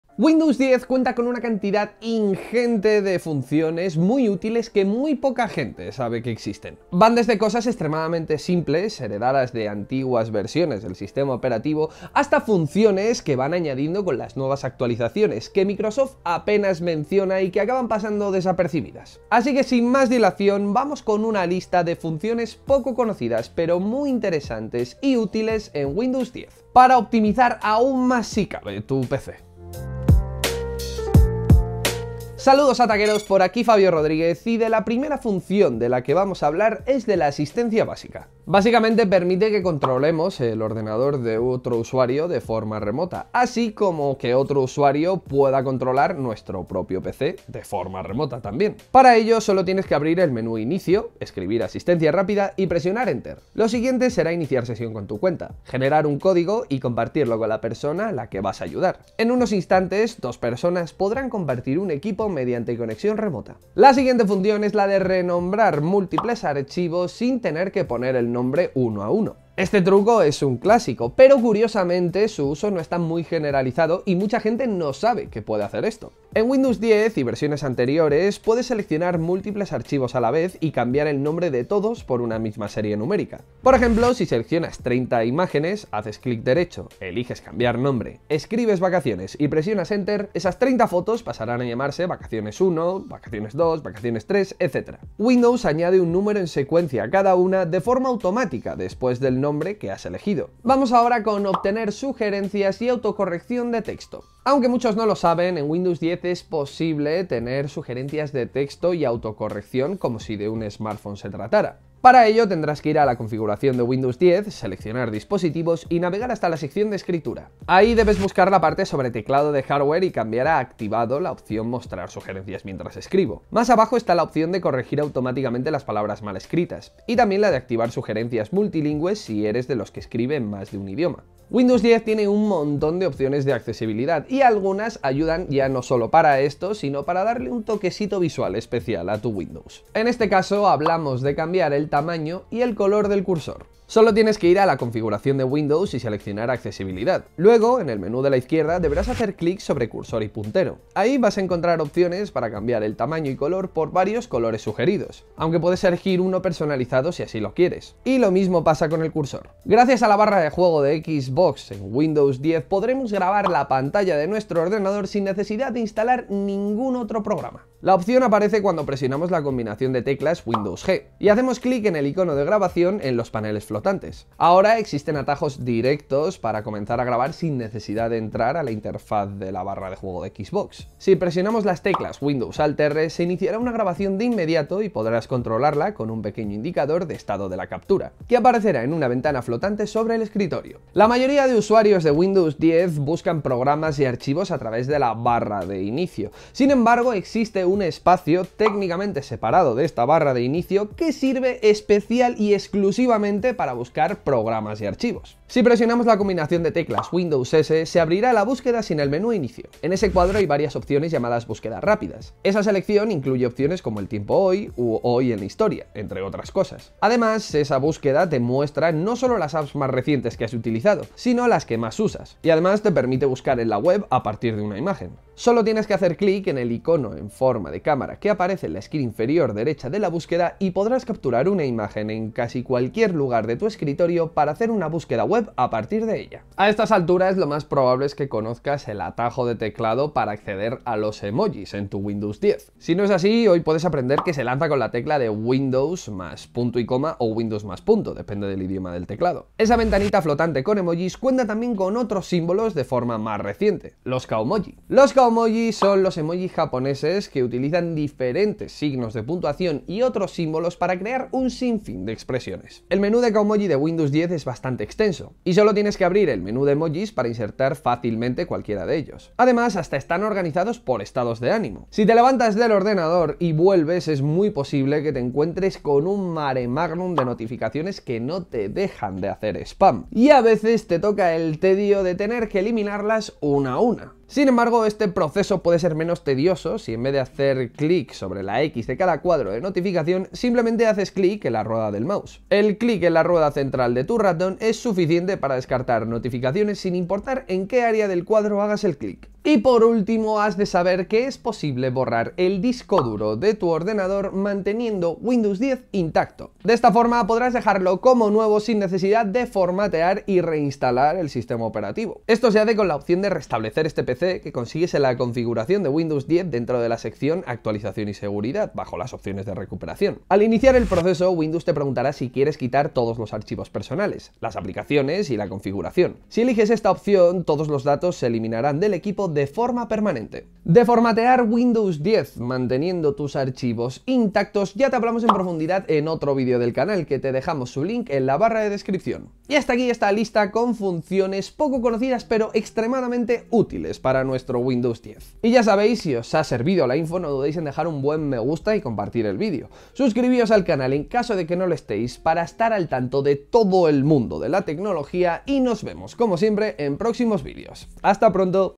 WINDOWS 10: FUNCIONES Y TRUCOS MUY ÚTILES y POCO CONOCIDOS
Windows 10 cuenta con una cantidad abrumadora de funciones útiles que muy poca gente sabe siquiera que existen. Van desde cosas extremadamente simples heredadas de versiones viejas de Windows, hasta novedades que se introducen con las actualizaciones más recientes, que Microsoft apenas menciona y terminan pasando desapercibidas.
Así que sin más dilación vamos con una tanda de funciones poco conocidas pero muy útiles para Windows 10 que pueden hacernos la vida más fácil, aumentar nuestra productividad, o simplemente alegrarnos el día frente al ordenador.

TRUCOS:
0:39 - Asistencia Rápida
1:06 - Renombrar archivos
1:43 - Sugerencias y autocorrección de texto
2:20 - Cambiar color y tamaño del puntero
2:43 - Grabar Pantalla
3:19 - Búsqueda Rápida
4:14 - Kaomojis
4:38 - Elimina notificaciones fácil
5:04 - Formatea tu PC manteniendo Windows 10